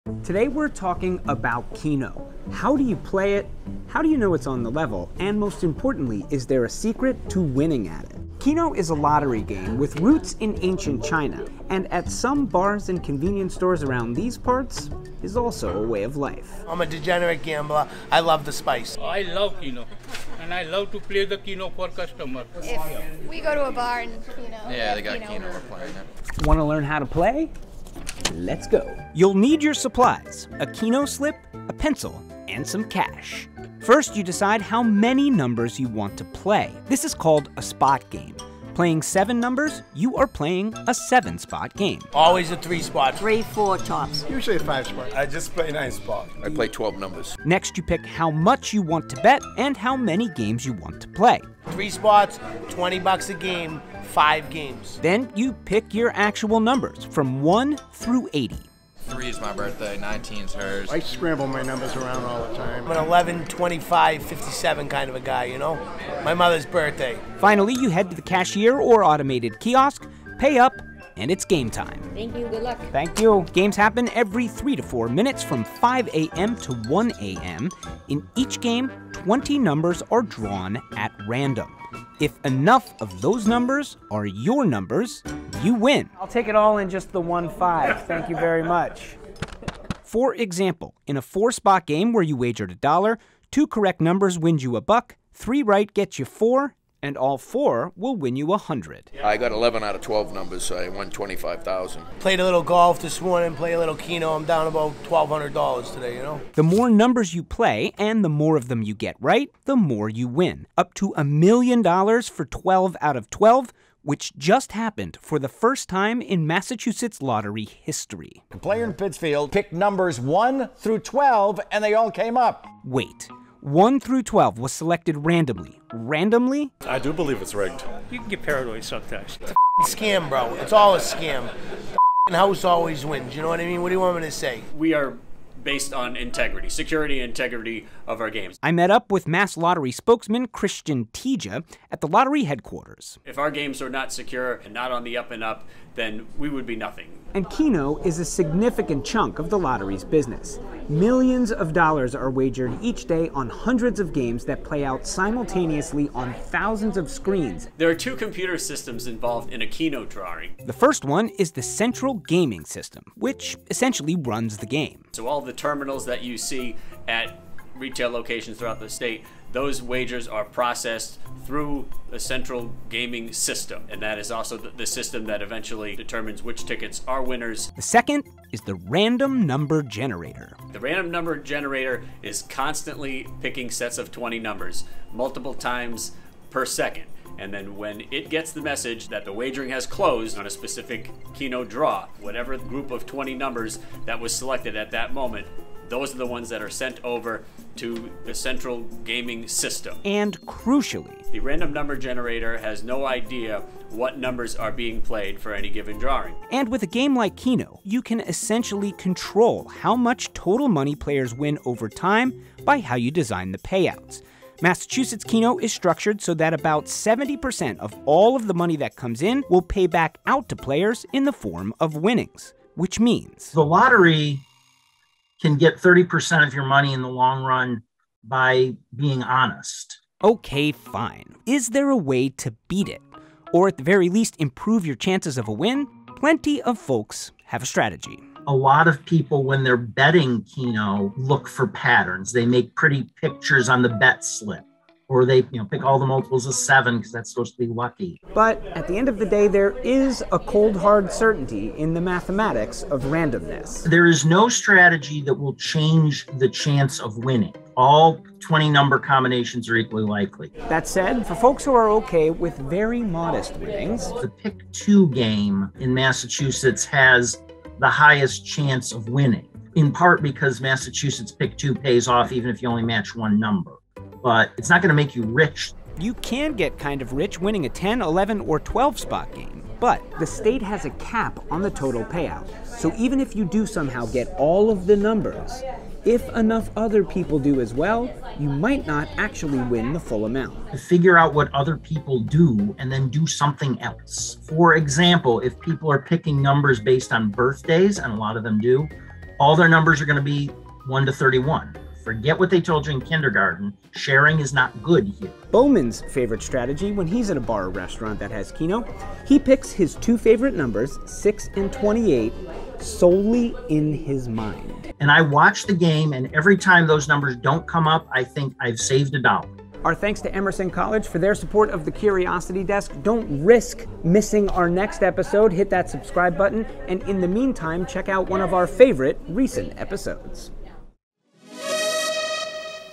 Massachusetts Keno: Can you actually win? | The Curiosity Desk
In this episode we take you inside the world of Massachusetts Keno. How do you play it? How can you be sure its on the level? And – perhaps most importantly – is there a secret to winning? On our quest for answers we visit Boston area bars and corner stores to talk with regular players, stop by the Mass Lottery HQ in Dorchester, and talk with a math whiz about Keno statistics and strategies.

Something you are curious about? Let Edgar know

We can’t do it without you. Support our award-winning community journalism by donating today